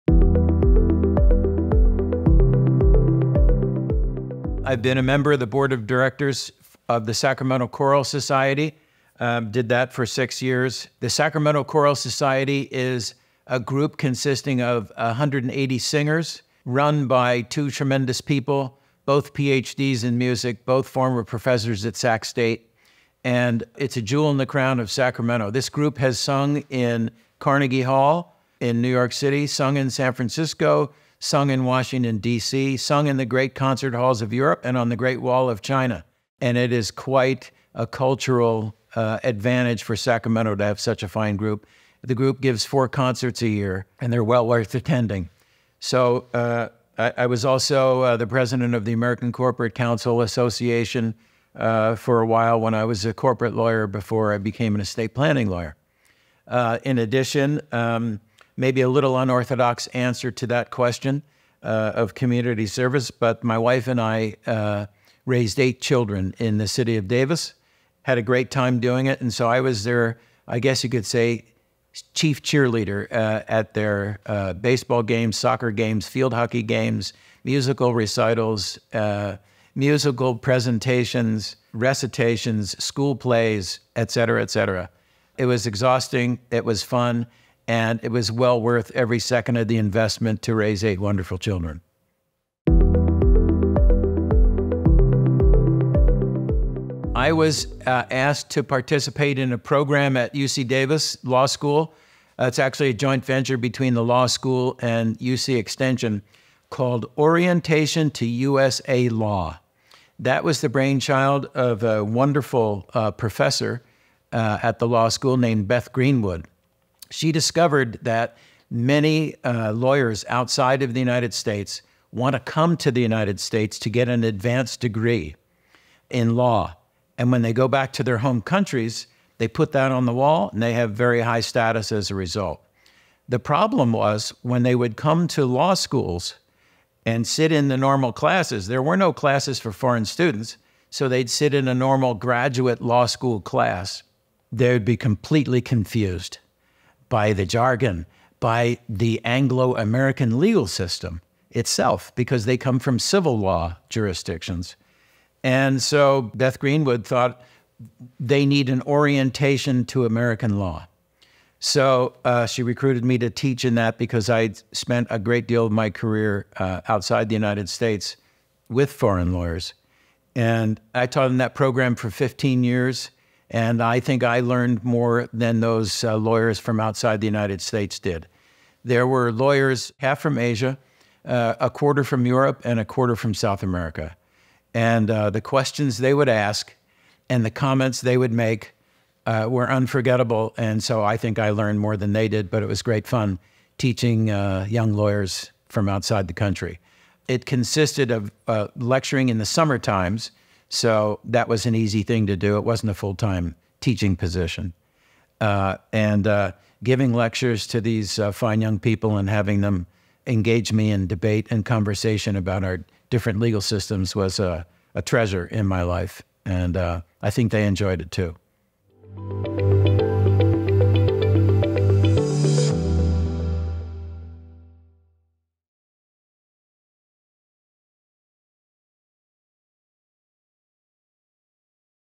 Meet R. Keenan Davis
Can you tell me about any scholarly lectures or writings you’ve done?

R. Keenan Davis serves as Senior Counsel at Kaminski Law Group and holds membership in the State Bar of California. Additionally, he is an English Solicitor with inactive status in the Law Society of England and Wales.

With a career spanning over thirty years, Keenan is a seasoned lawyer, university-level teacher, and hands-on mentor. He has represented start-ups, high-growth companies, and small business owners, forming close relationships with clients who become friends. Keenan takes pleasure in assisting them with their legal matters, including succession planning for the orderly transfer of ownership to partners, including the next generation.

During his fifteen-year tenure as General Counsel for System Integrators, Inc., a notable Sacramento high-tech success story, Keenan played a pivotal role in various strategic initiatives. This included involvement in private round equity and debt financings, an Initial Public Offering, a management-led leveraged buyout, intellectual property matters, SEC regulatory compliance, and numerous mergers and acquisitions.

As a lead negotiator, Keenan facilitated transactions worldwide, overseeing negotiations for large-scale technology licensing agreements totaling over a billion dollars in the U.S., Europe, and Asia. His extensive travels accumulated over a million air miles in the process.